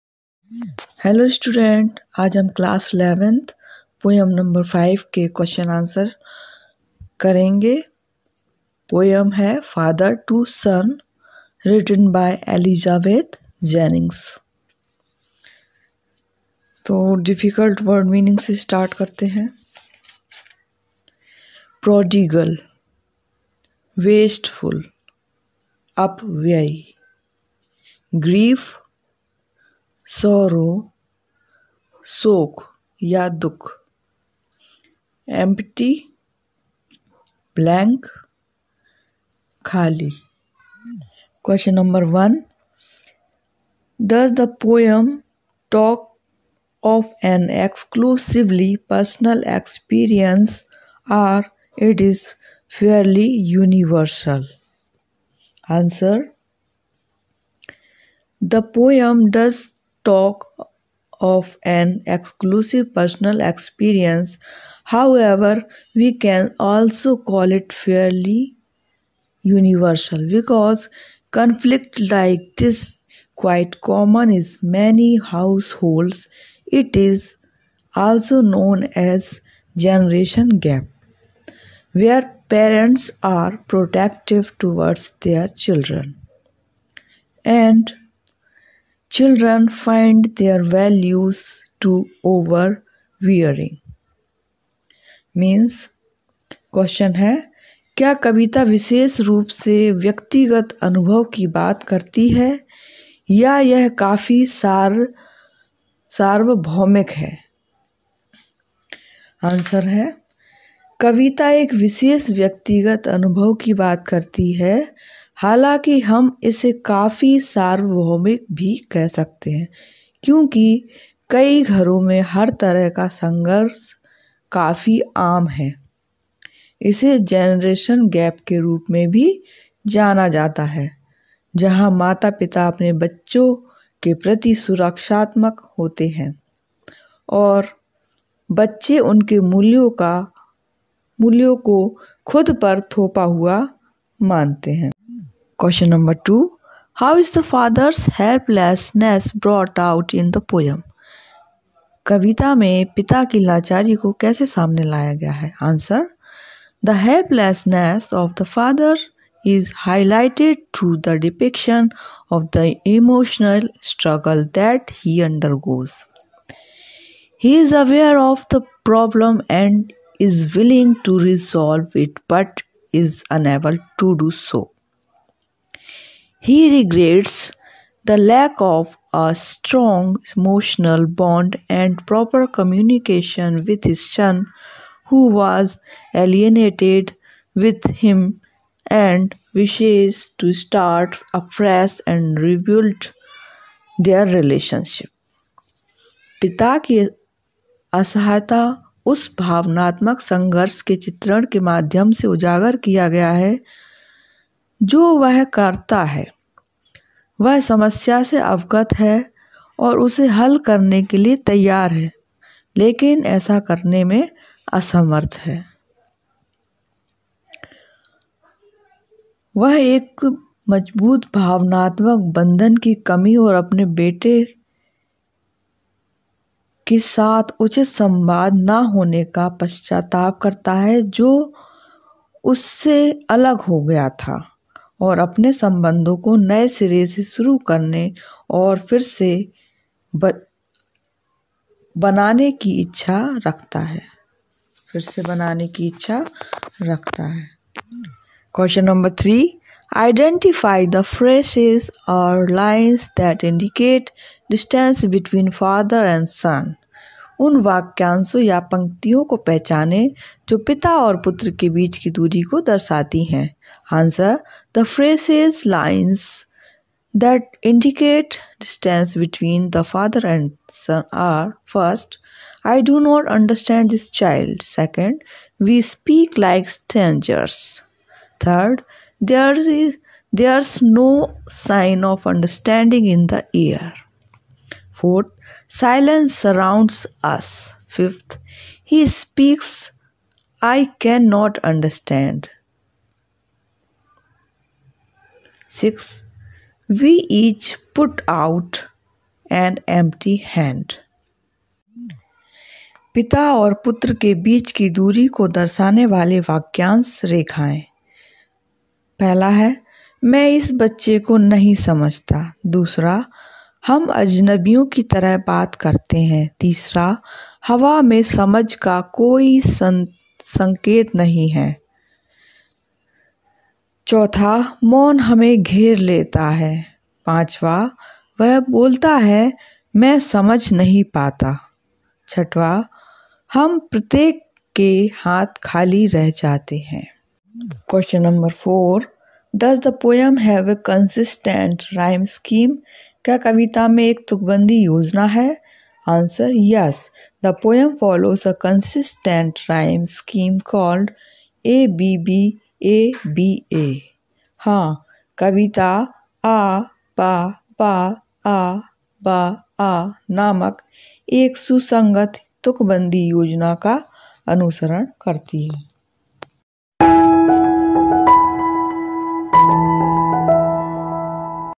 March 8, 2021कक्षा 11#English -Poem-Father to Son Questions and Answers Difficult Word meaning
Difficult Word Meaning
by Savitri Prabhakar